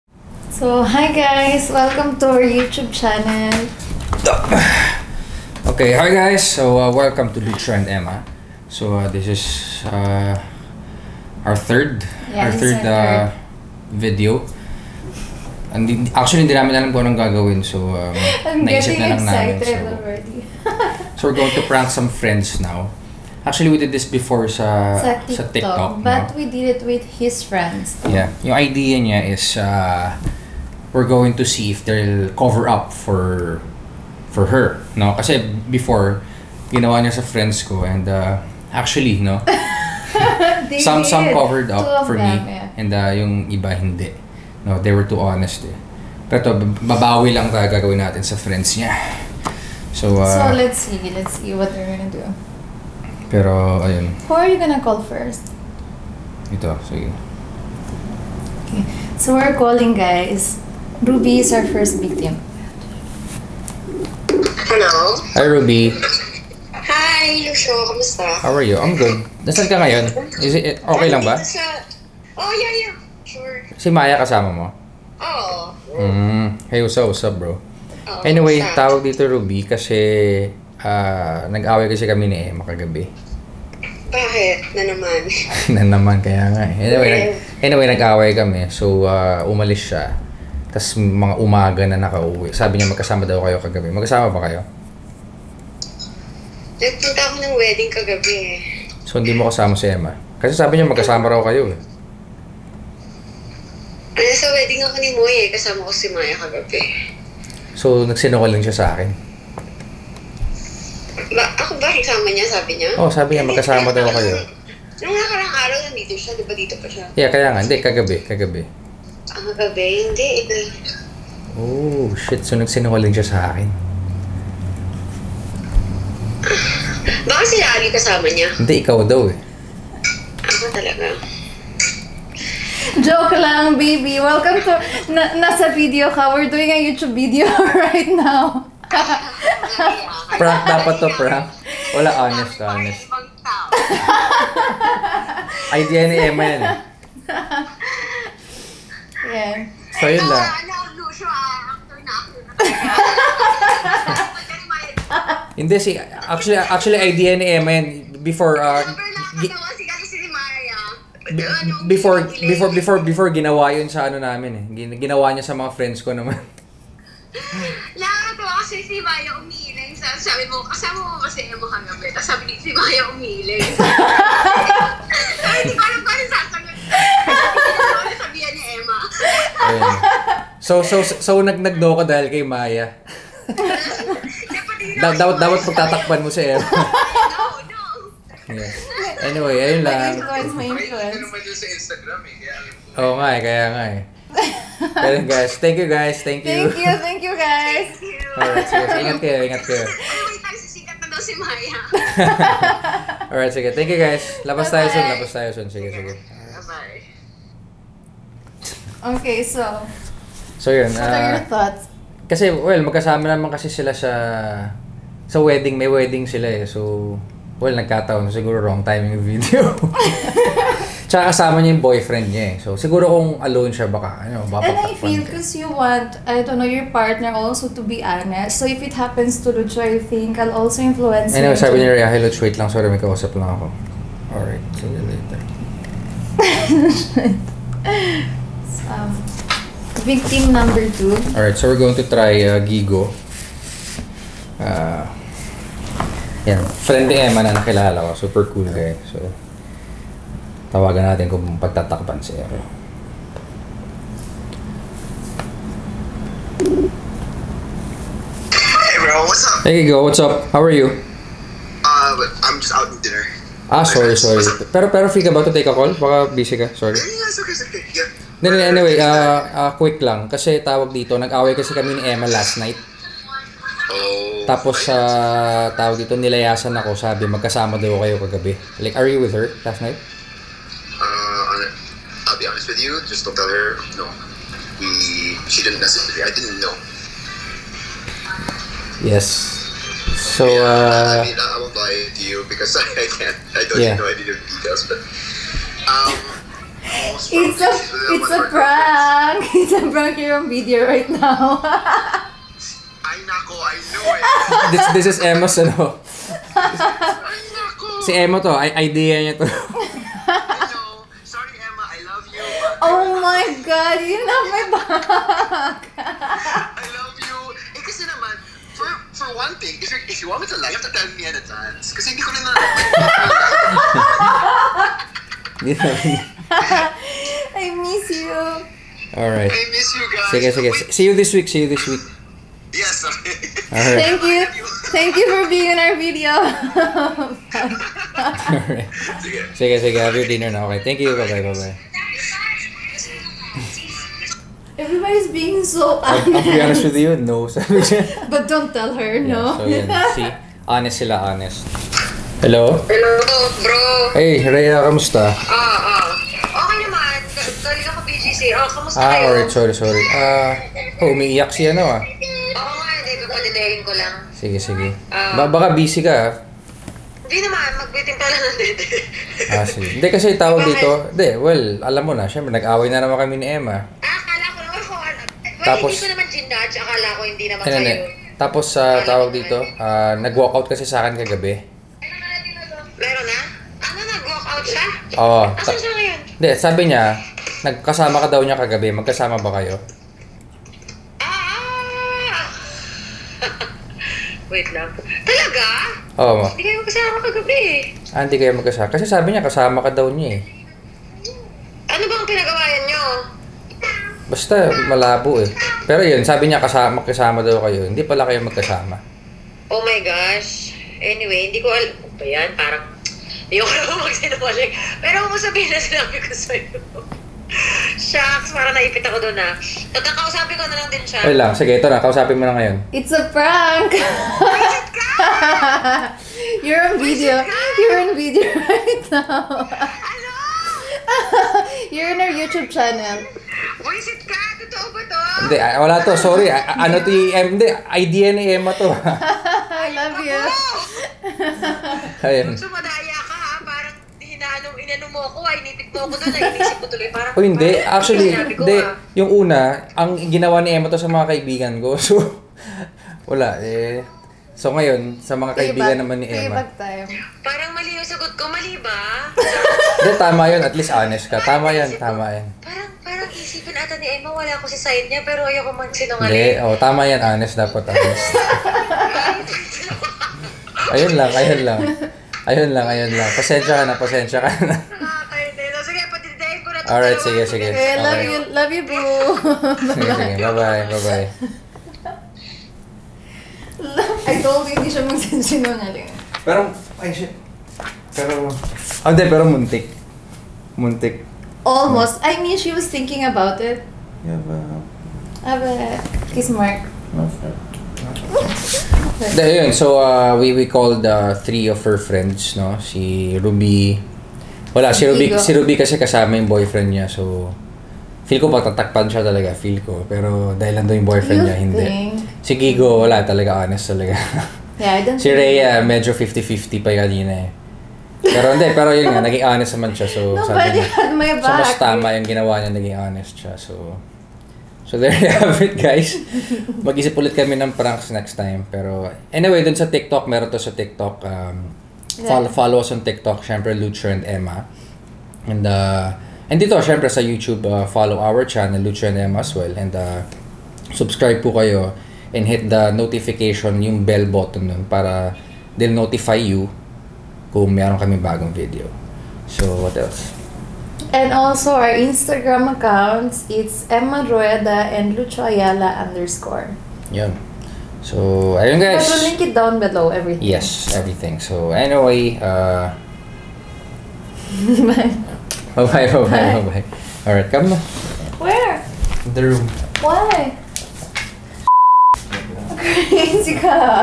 Prank Calling Our Friends To See If They Will Cover For Us | *Loyalty Test*
The first version of this was uploaded on Tiktok wherein Emma "prank" called some of my friends. Now it's time for some revenge! Haha. We tried it on Emma's friends too.